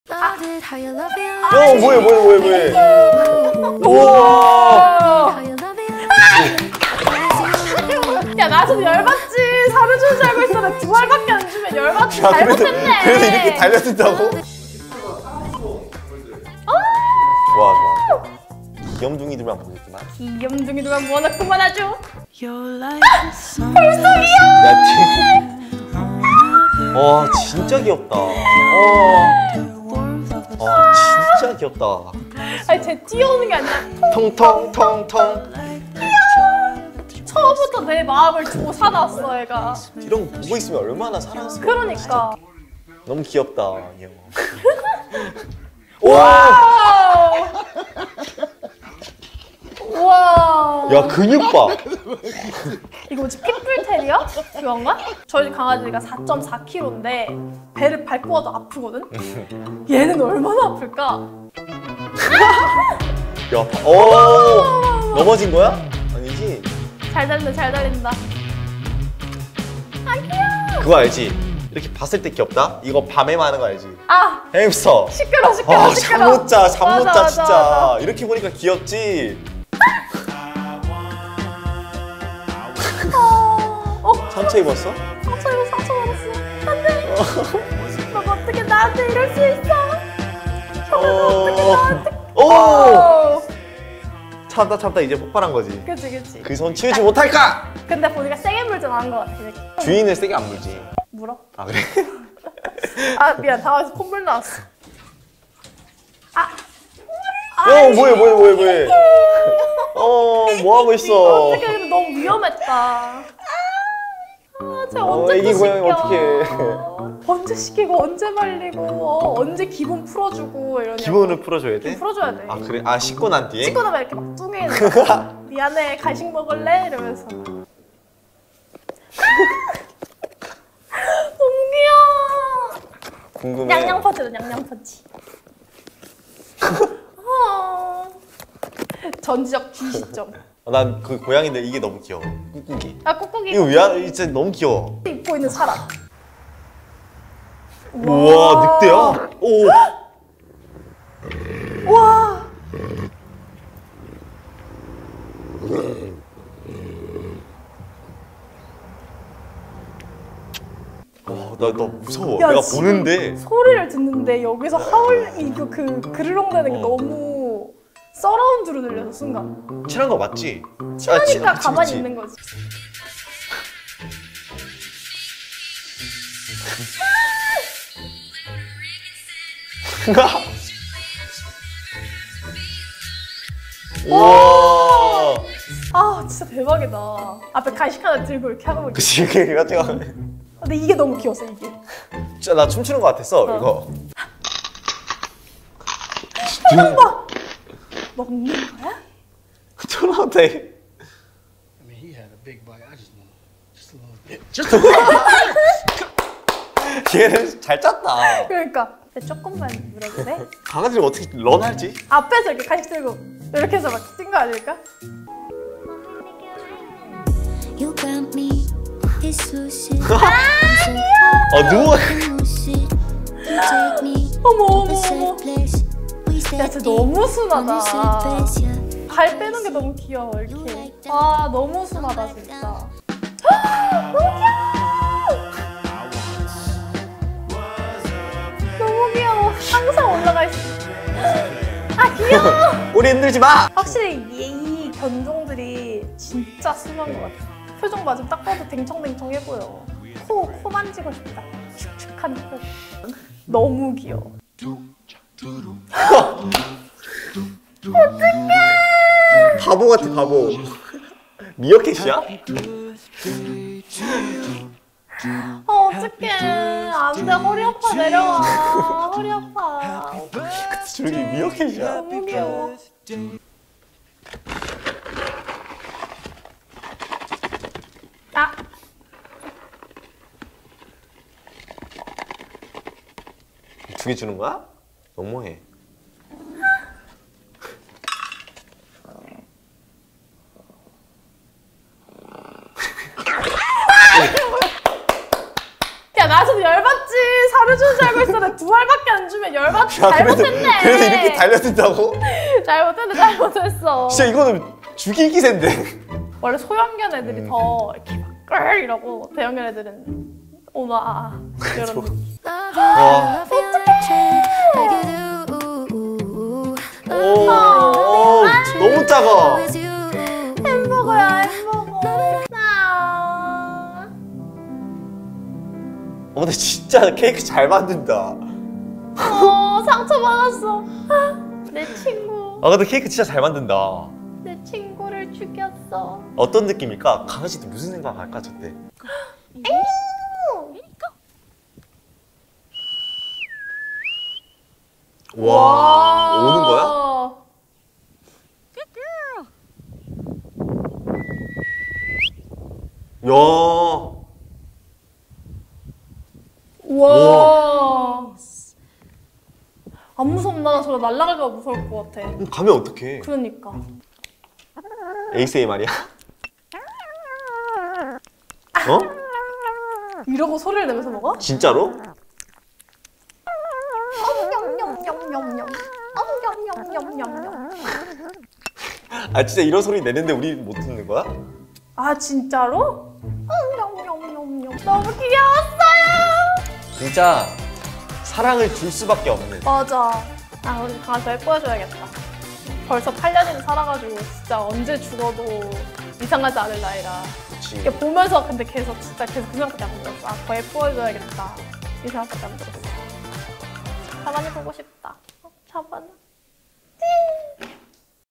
틱톡 ‘Adorable Pets’를 처음 본 한국인 남녀의 반응 | Y
So happy 😍